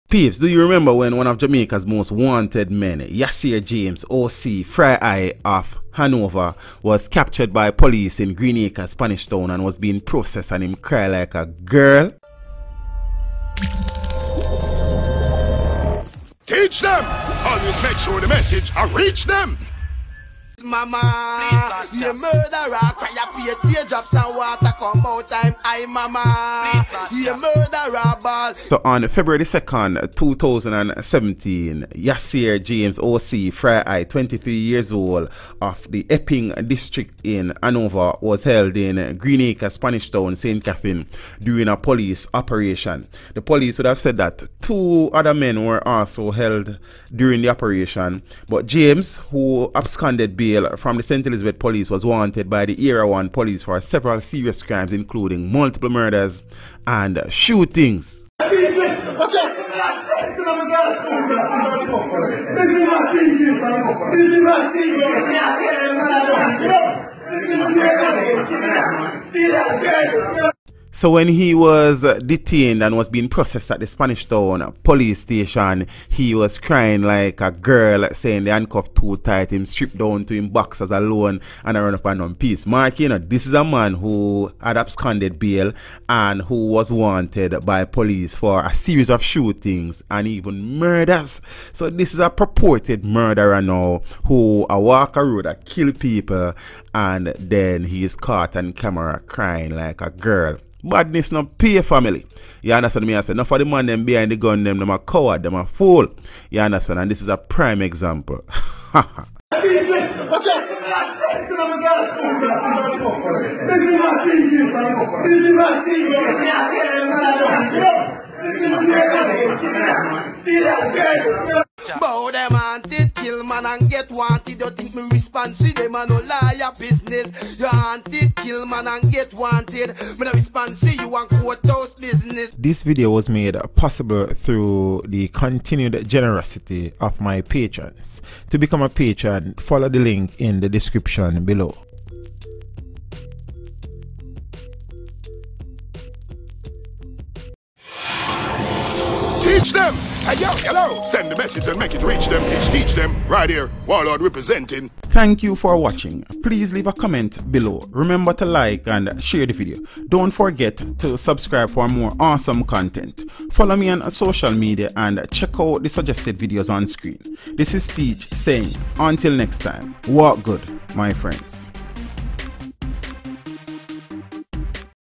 Do You Remember When? Jamaica's MOST WANTED Man CRIED like a BABY - Teach Dem
Arrested 23-Year-Old Linked To 10 Murders And Counting - Cops

One of Jamaica’s most-wanted men, 23-year-old Yasser James alias 'Fry Eye' has been captured.

James who is from Epping district, Hanover was held in Green Acres, Spanish Town, St Catherine during a police operation this morning.

The police say two other men were also held but their identities are being with withheld pending further investigations.

James, who absconded bail from the St Elizabeth police was wanted by the Area One Police for several serious crimes including multiple murders and shootings.

This morning's operation involved the Counter Terrorism and Organised Crime Branch, the Narcotics Division and the St Catherine North Police.